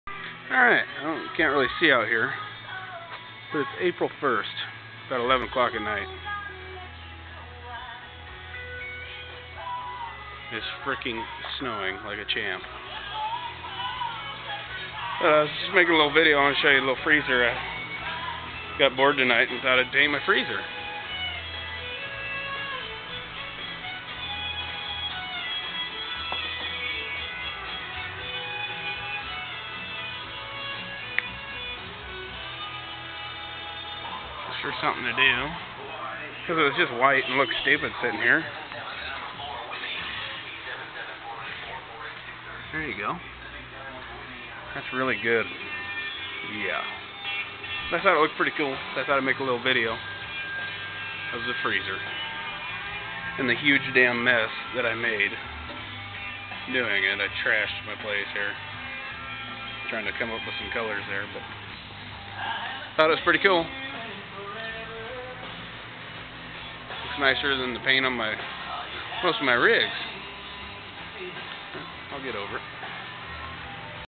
custom painted freezer
Got bored and painted my shop freezer